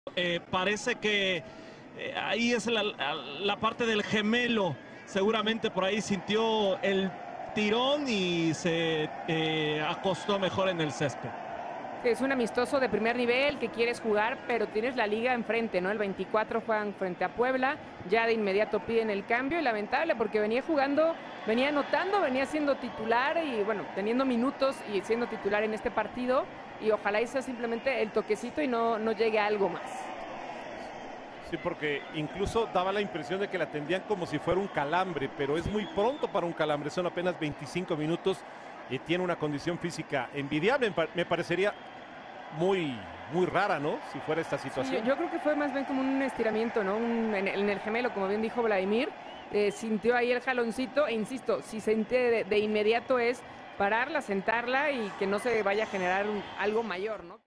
¡UNA LÁSTIMA! Kanu no puede seguir | Tigres 1-0 Bayern | Amistoso Internacional Femenil | TUDN
La nigeriana se recuesta tras sentir dolencias en su gemelo derecho, por lo que es reemplazada por Belén Cruz.